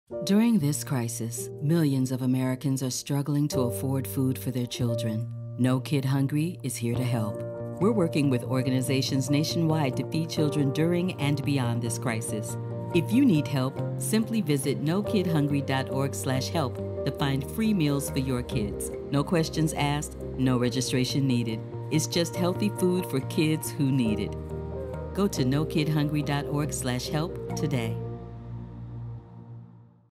Free Meals For Kids – Visit NoKidHungry.org/help
No Kid Hungry is working with organizations nationwide to feed children during and beyond this crisis. If you need help, simply Visit NoKidHungry.org/help to find free meals for your kids.